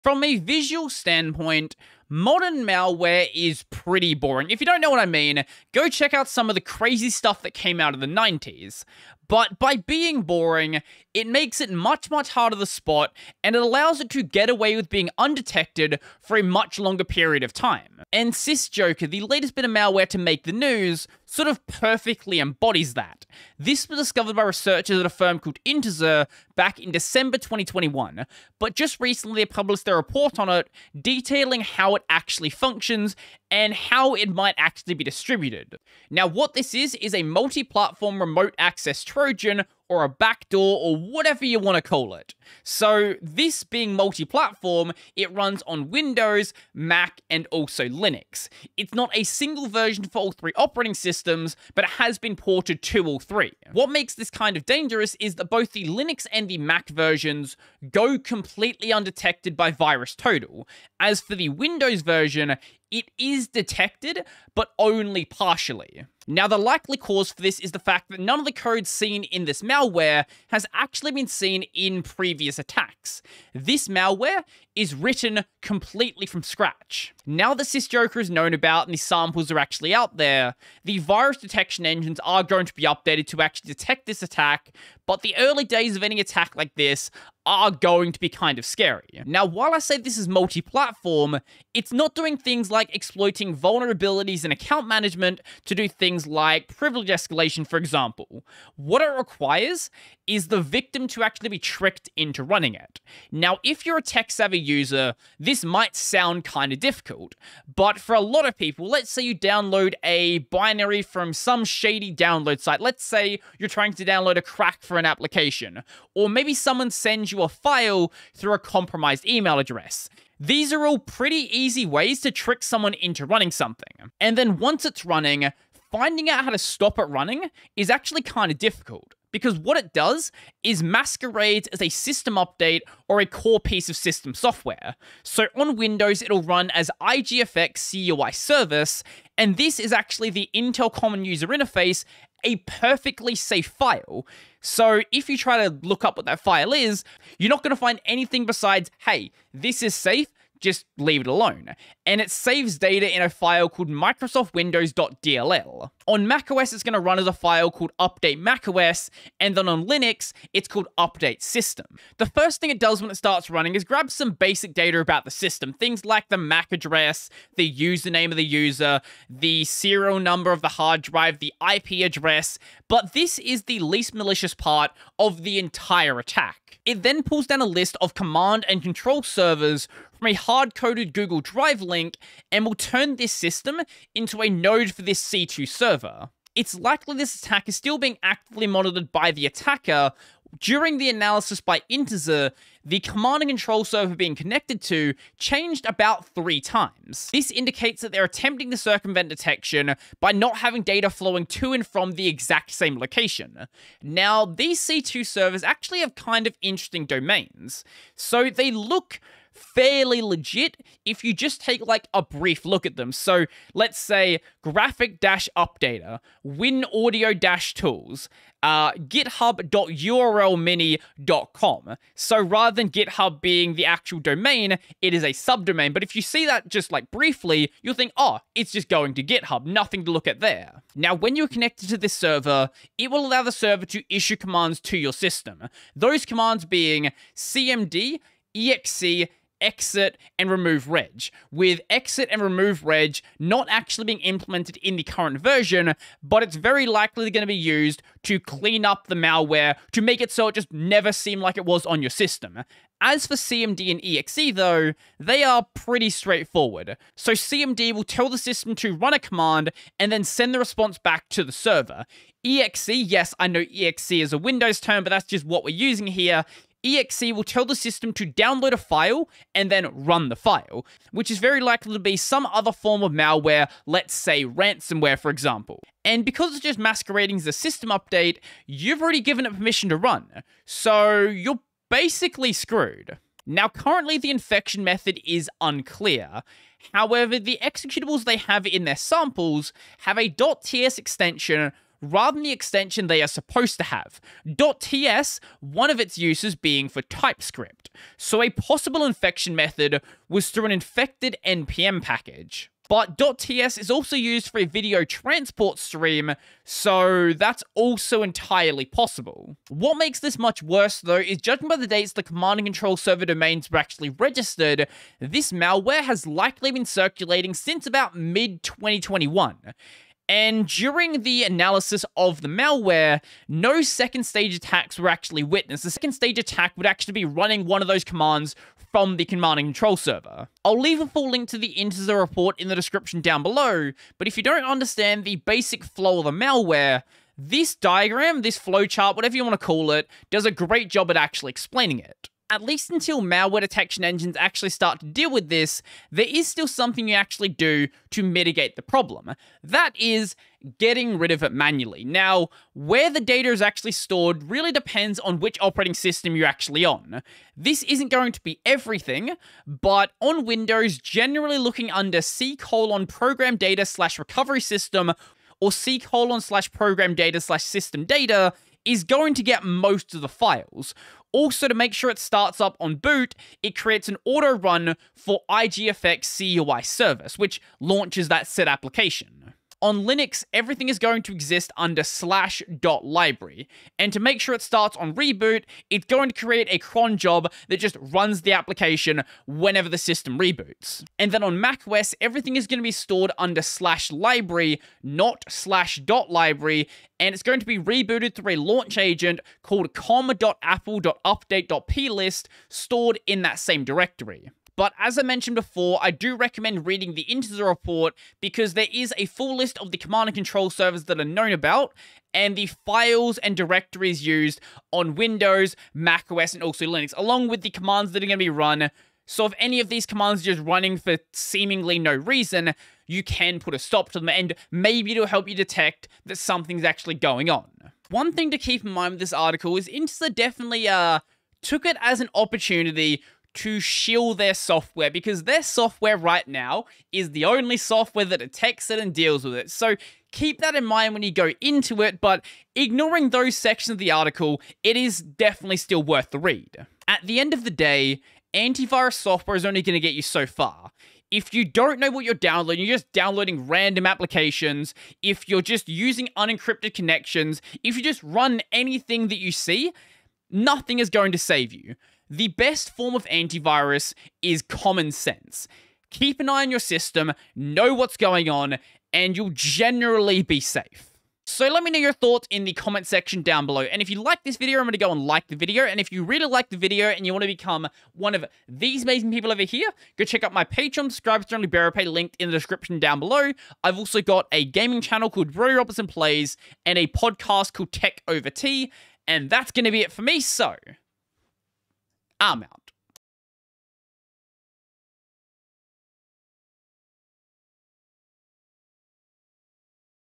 This Malware Backdoor Hides In Plain Sight
Modern malware is boring but by being boring it can hide in plain sight, this is Sysjoker the malware that hides as a system update and runs on all 3 major updates

Time Stamps
0:00 Latest Malware
1:29 How It Works
5:42 How To Deal With It
7:27 Common Sense Anti Virus

Credits
🎨 Channel Art:
All my art has was created by Supercozman